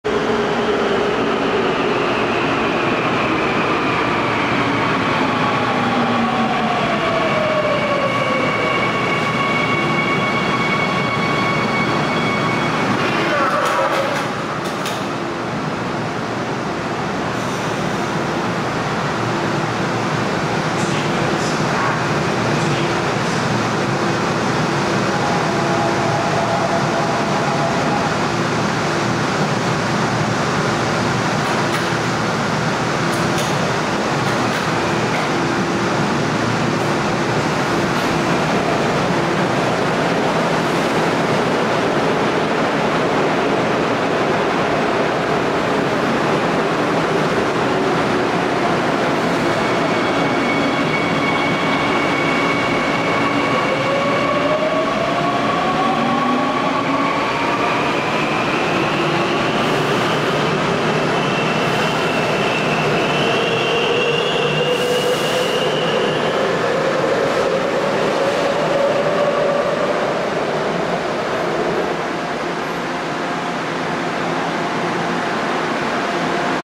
Seoul Metro Line 2 train at Hanyang University
Seoul Metro Line 2 Inner Circle Line train of Hyundai Rotem Seoul Metro 2000-series (VVVF trainset 2x63) arriving at & leaving Hanyang University. The screen doors were left open for inspection, providing a nice view.
서울 메트로 2000호대 (VVVF 2x63편성) 2호선 내선 순환 열차 한양대역을 도착와 출발합니다.
This video was taken on August 9, 2015.